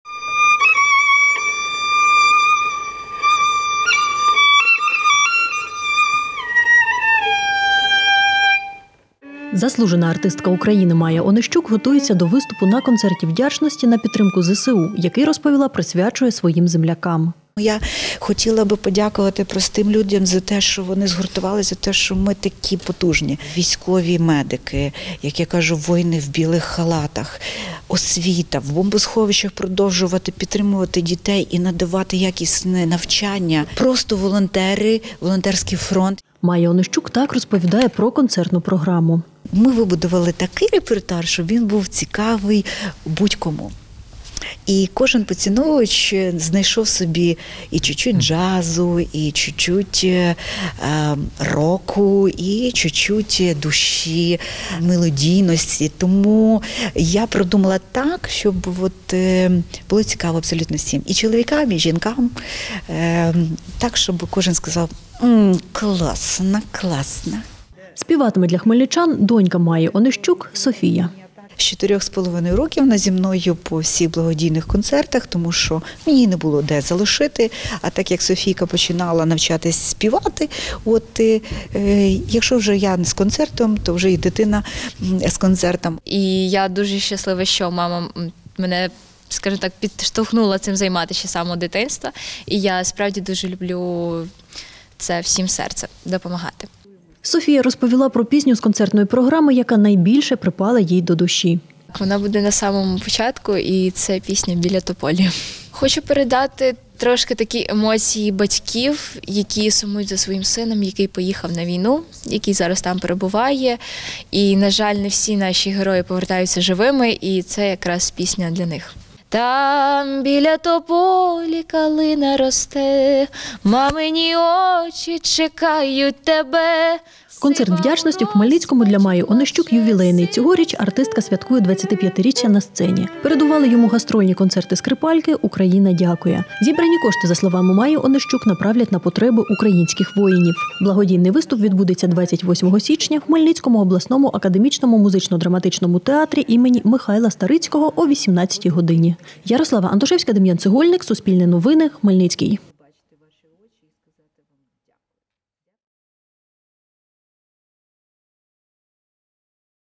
Концерт - подяка на підтримку ЗСУ відбудеться у Хмельницькому
Концерт-подяка на підтримку ЗСУ відбудеться цієї суботи в обласному центрі. Для глядачів виступить заслужена артистка України Майя Онищук з донькою Софією та оркестром Хмельницького обласного академічного музично-драматичного театру імені Михайла Старицького. Що підготували артисти для глядачів – знає команда Суспільного.